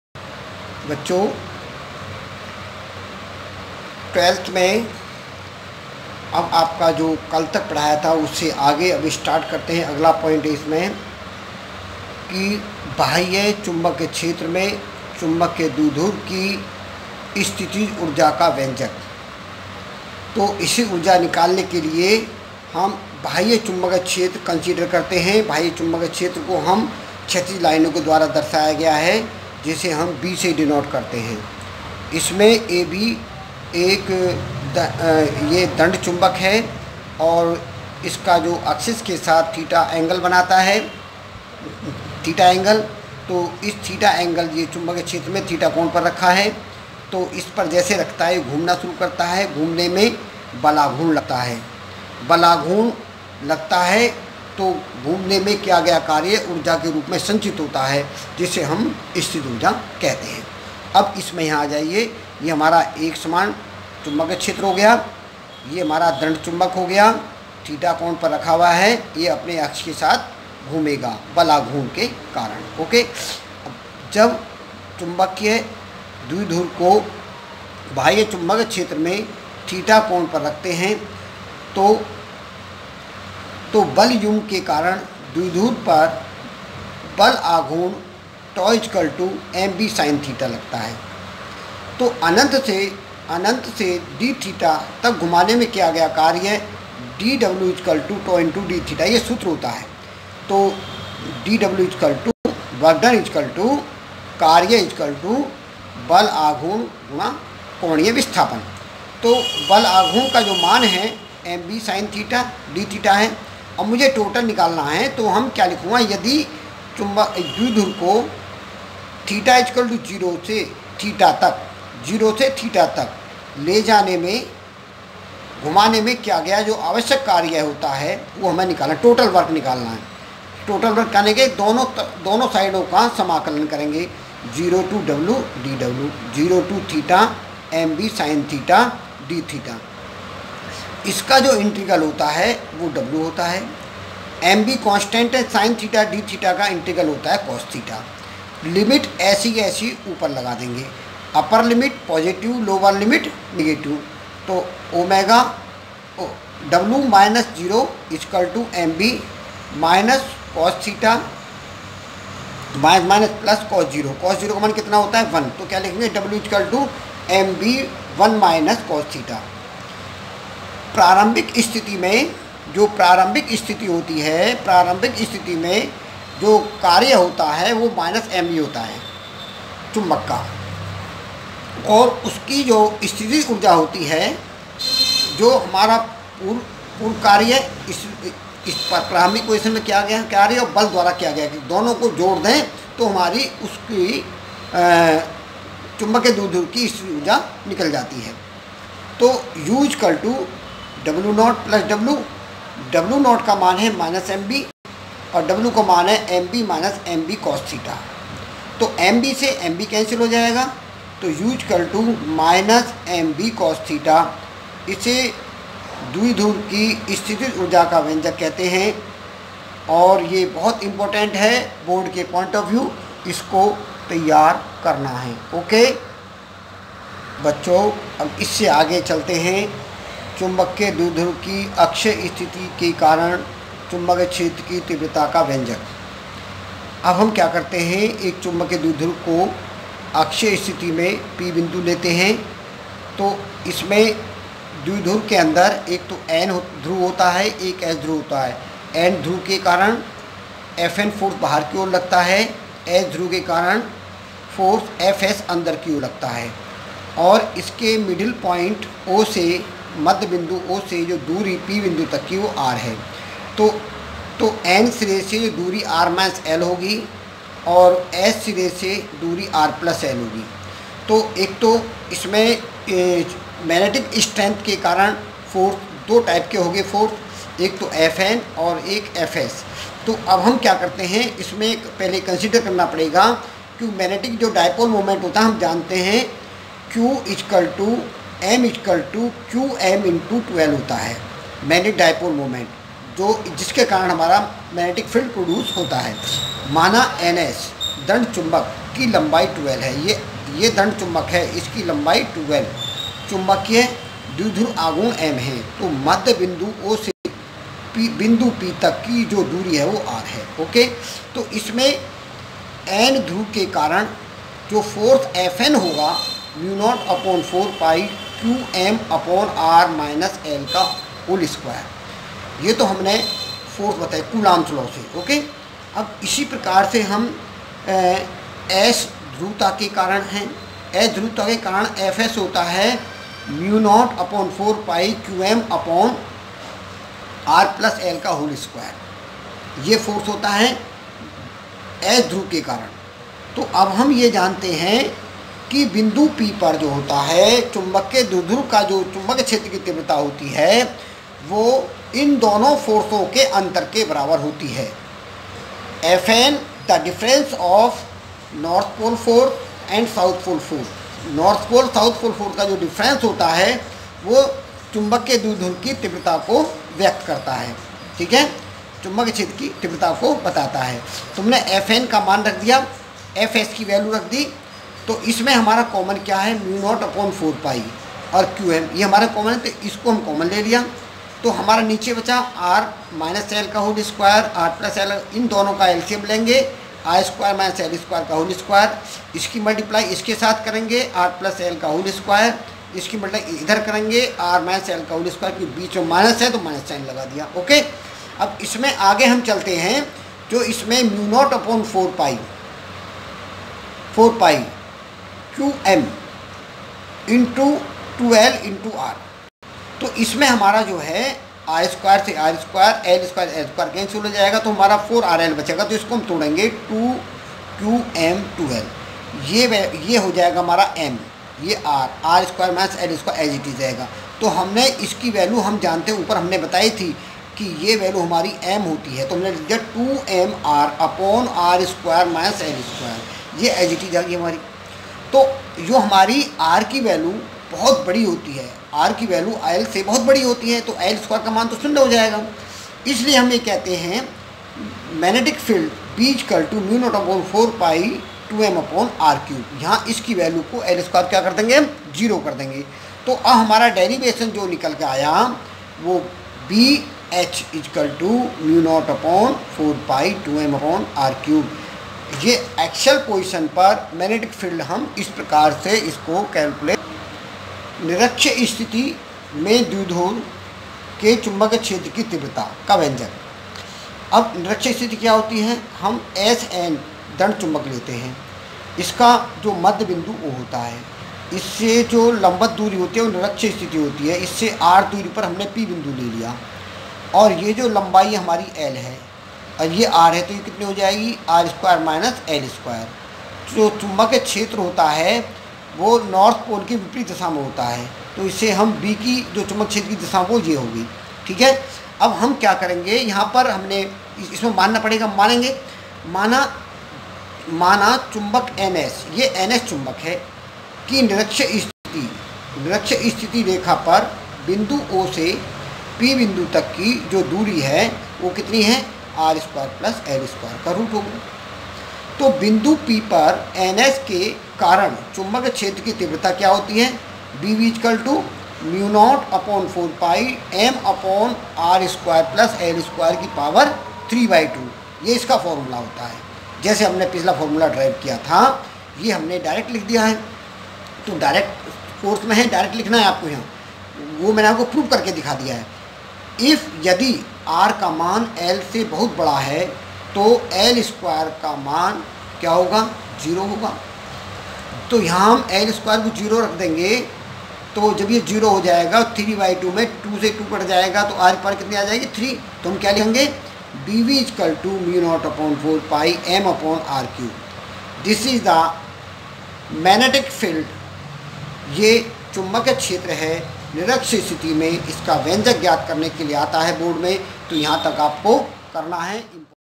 Part1- Magnetic field due to bar magnet, Part2- Magnetic force between two dipole in HINDI
Class- XII
UP Board
PS Tomar